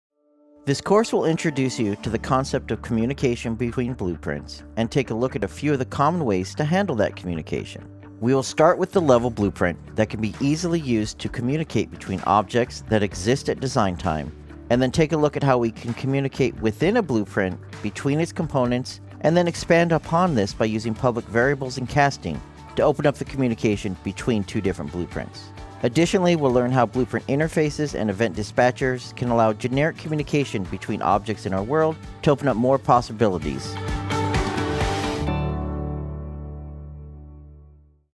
Blueprint Communication Preview
In this course from the Epic Online Learning team, you'll learn how to create more efficient projects by dividing and organizing your Blueprints by functionality.

After taking this course you will be able to:

-Define the different types of Blueprint Communication in Unreal Engine.
-Apply different methods of Blueprint Communication to an Unreal Engine project.